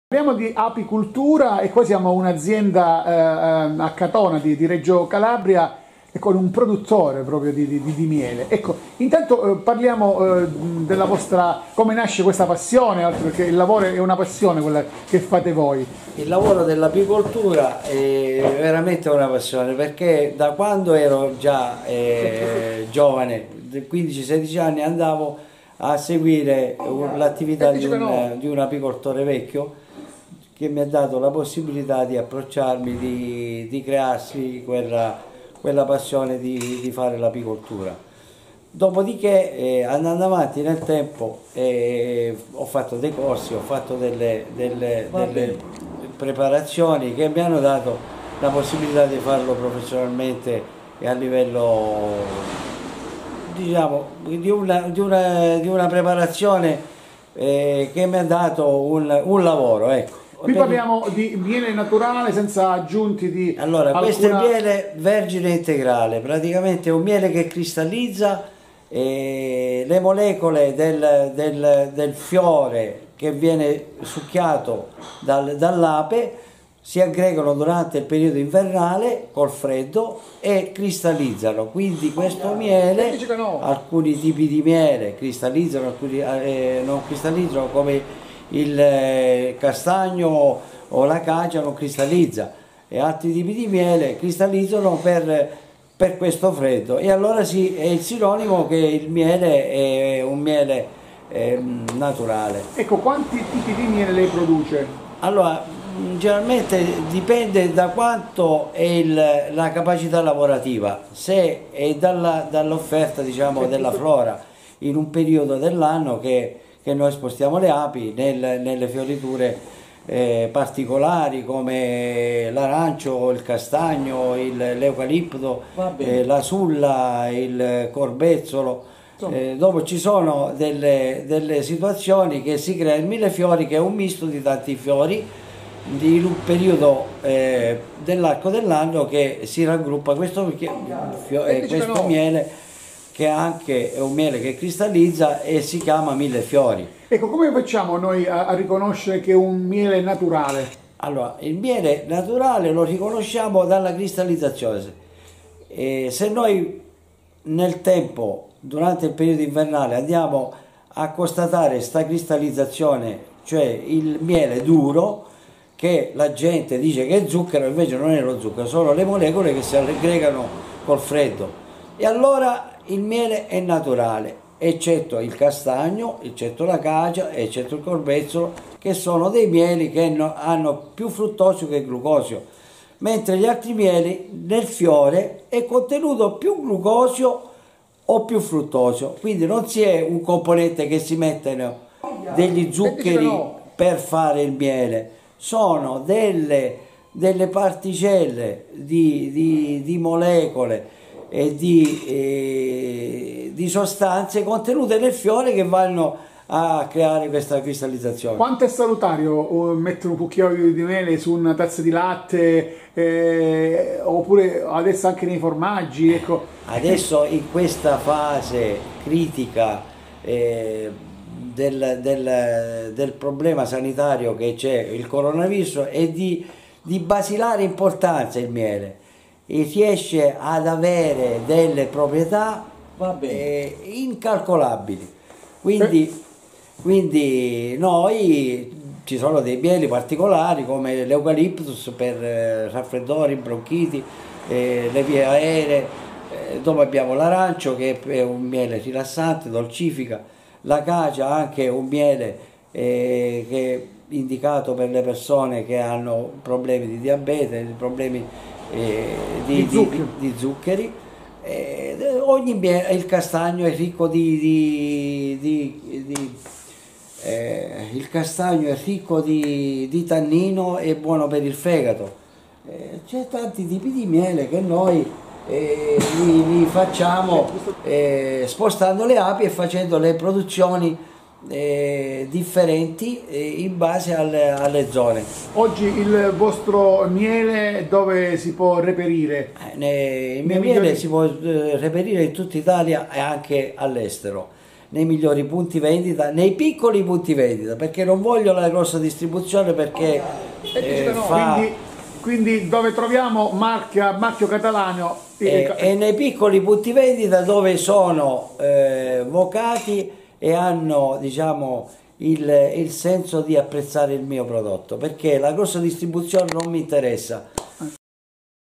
Reggio Calabria: il miele grande alleato contro le malattie, sentiamo apicoltore di Catona.
Reggio Calabria: il miele grande alleato contro le malattie, abbiamo ascoltato un apicoltore e agricoltore della zona di Catona.